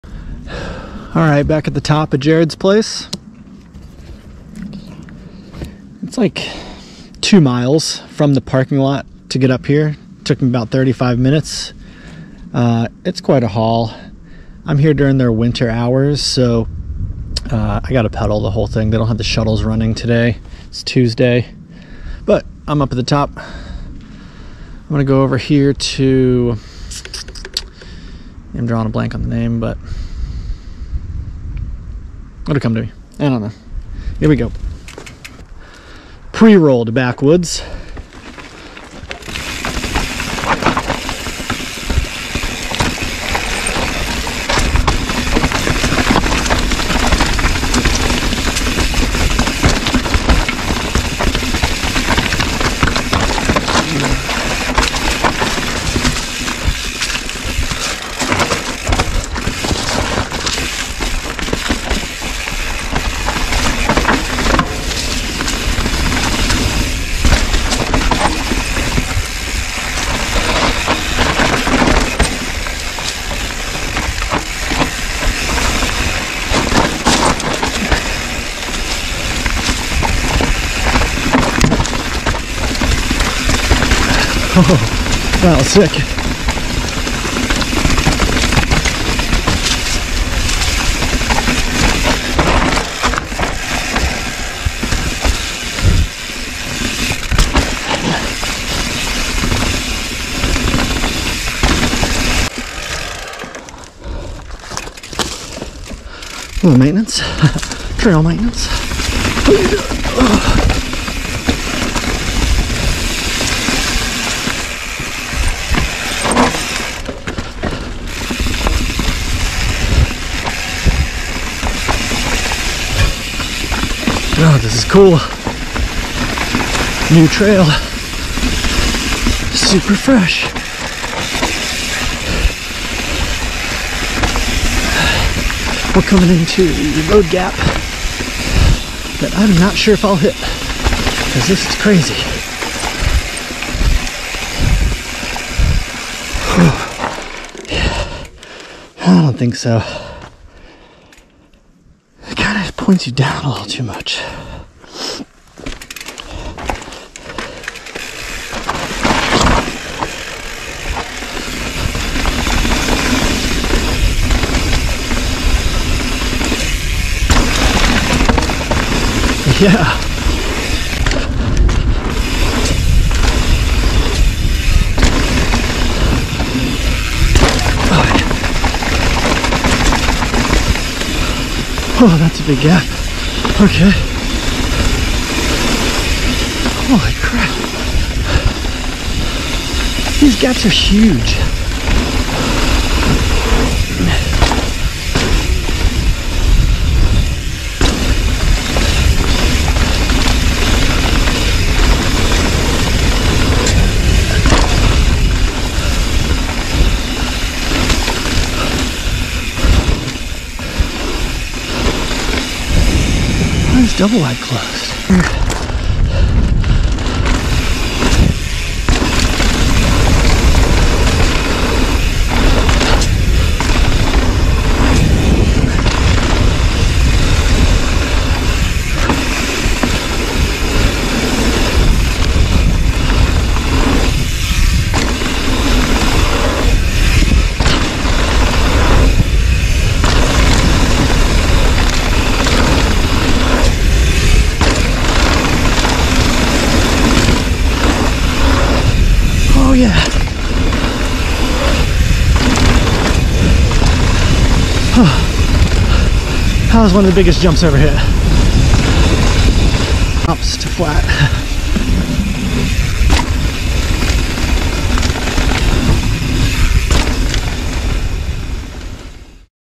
New Trails at Jarrod’s Place. Preroll into Backwood!
This ride starts at the top of the mountain. Taking Pre Roll into Backwood. Backwood is a new single black trail with fun berms, drops and gap jumps over other parts of the trail. Mid way down I switch over to senior discount because the lower section of backwoods was pretty wet.